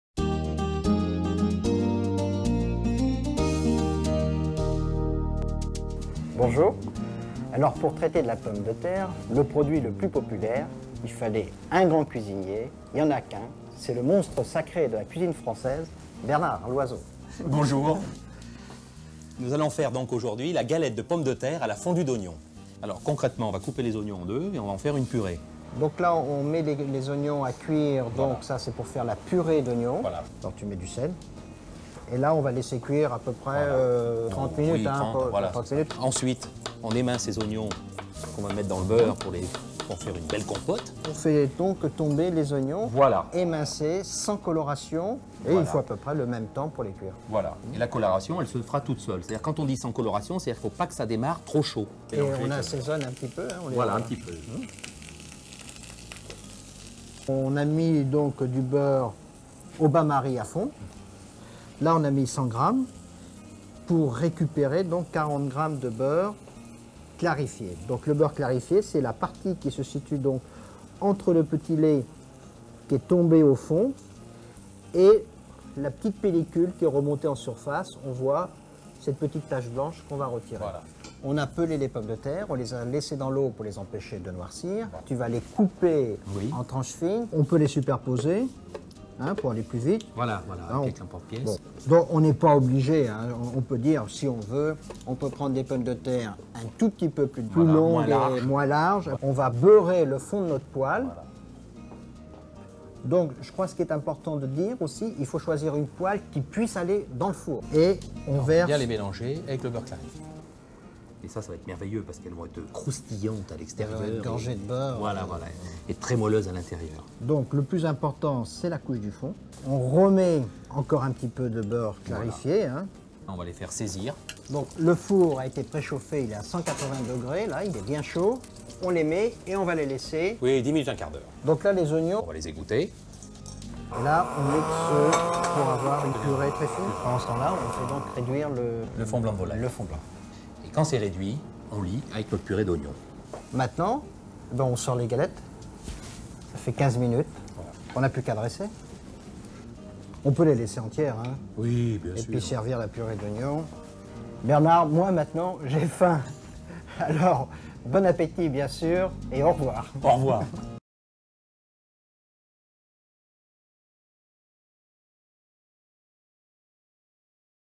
Galette de pomme de terre
Les galettes de pomme de terre à la fondue d'oignons  de Bernard Loiseau sont un excellent accompagnement

► Ingrédients pour 3 personnes :

4 grosses pommes de terre Charlotte
40 g de beurre clarifié
15 g de beurre
5 oignons moyens
2 dl de fond blanc de volaille
Sel et poivre du moulin